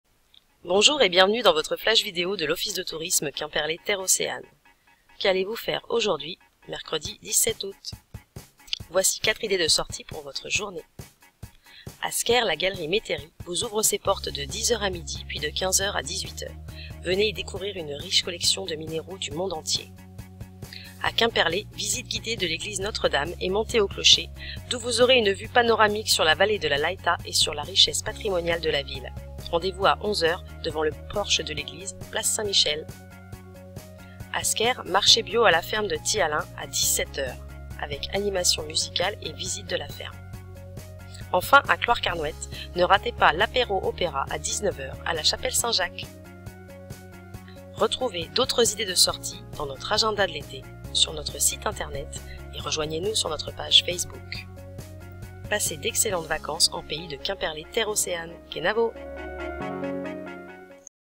Flash vidéo mercredi 17 août
Bonjour et bienvenue dans votre flash vidéo de l’Office de Tourisme Quimperlé Terre Océane.

Qu’allez-vous faire aujourd’hui ce Mercredi 17 Août ?

Dans cette courte vidéo, l’Office de Tourisme vous propose 4 idées de balades pour votre journée.

Retrouvez toutes les manifestations sur la page Agenda de notre site Internet et sur notre page Facebook.